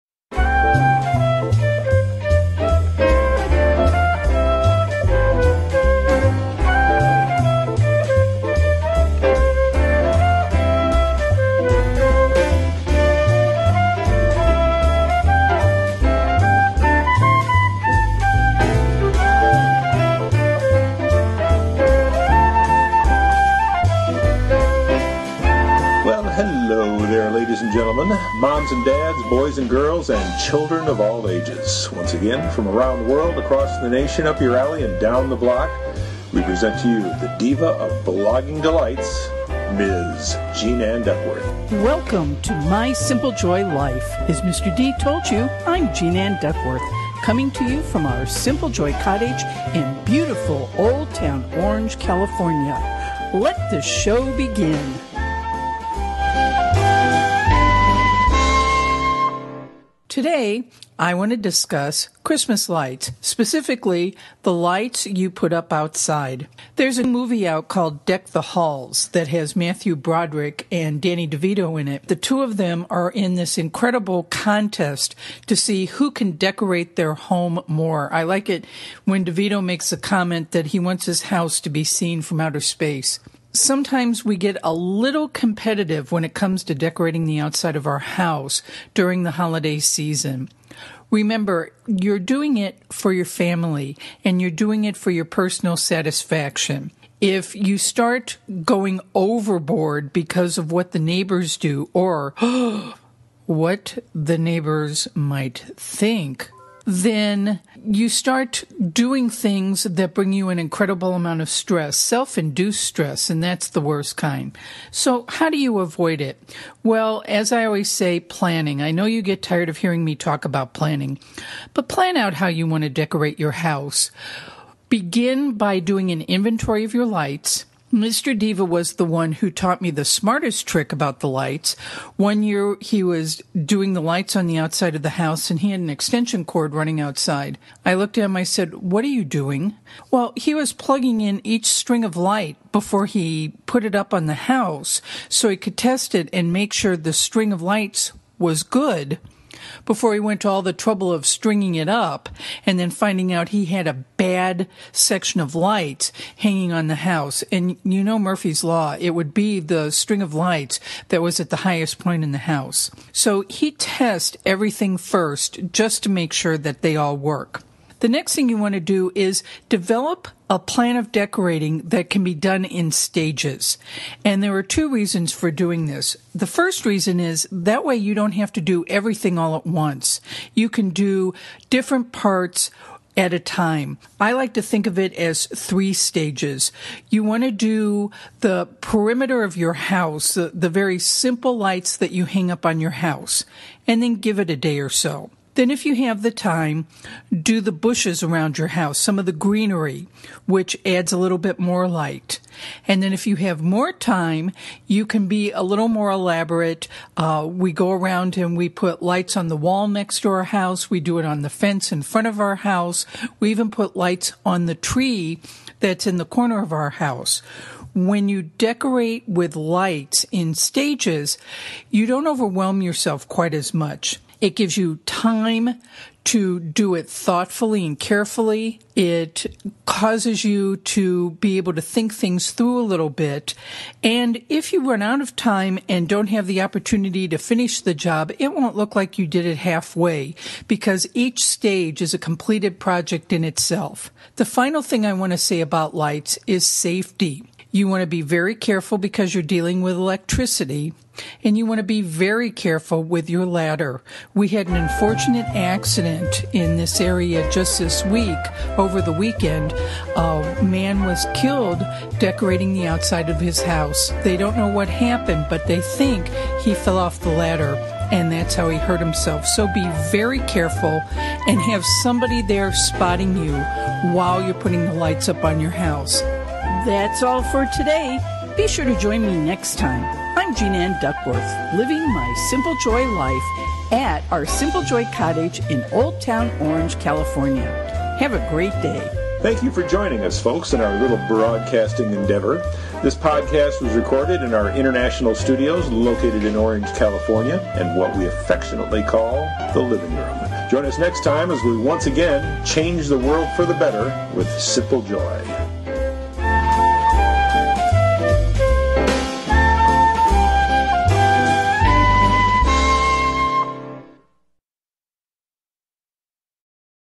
The Simple Joy of Holiday Decorating Part 3
Preparing for the Holiday season can be exciting! It can also be extremely stressful! There is so much to do on top of everything else you must get done each day, each week. How can you possibly add one more thing to your already busy schedule? Still, Christmas is coming whether you are prepared or not! This year, reduce Holiday stress when you take the Simple Joy apporach to Christmas!

This week, Jean Ann discusses ways to simplify the process of decorating your home for Christmas. You can have a beautiful home filled with Holiday decorations without the added stress. Each day this week, Jean Ann offers tips & ideas on a different area of Holiday home decoration!

This Christmas, let's take the stress out of the season! Let's make Christmas a Simple Joy!

For more tips & ideas to help you reduce Holiday stress, please